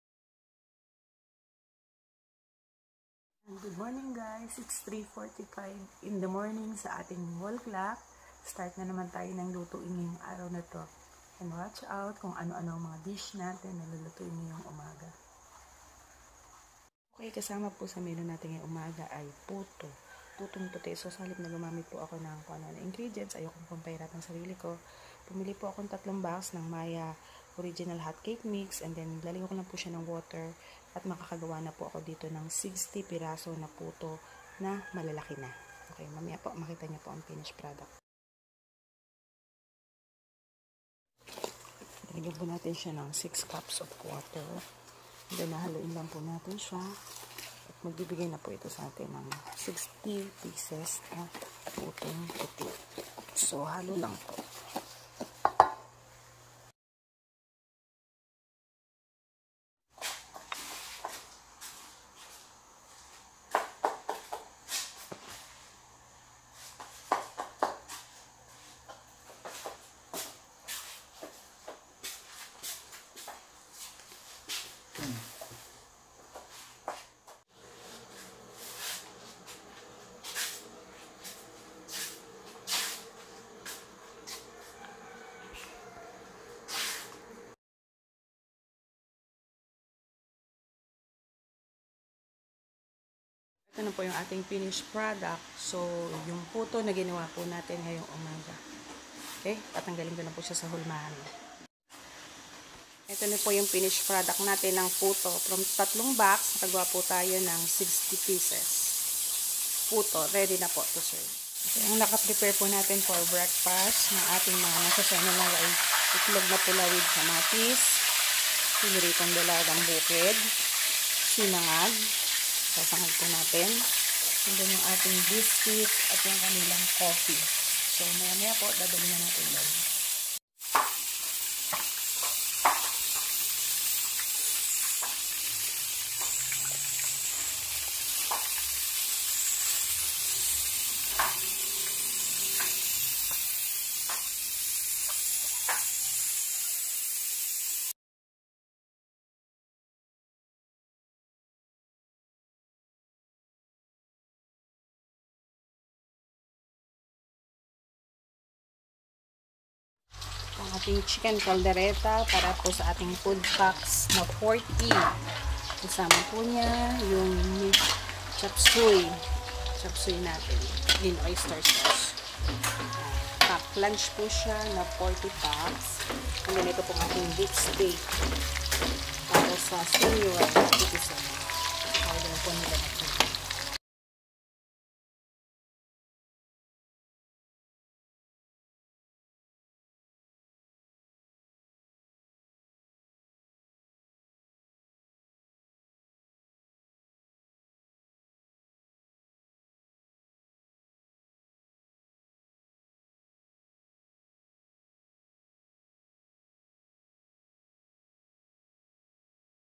Food Prepared Today
Second day of food preparation for seminar foodpacks and bday.